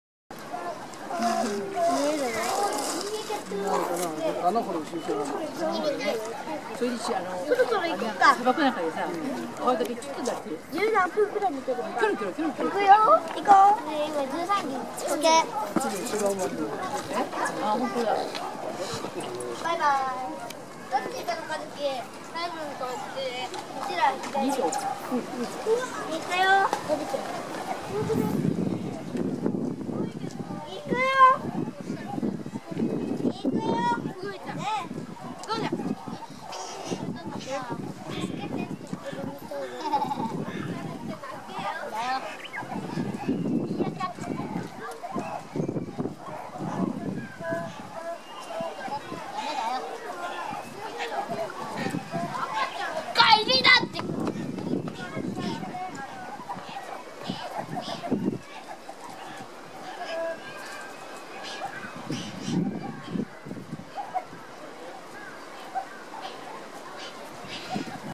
乗っかって
ズーラシアのミーアキャット。
寝ている子の上に、違う子が乗っかって山になっていました。
重そうだけど、暖かそう。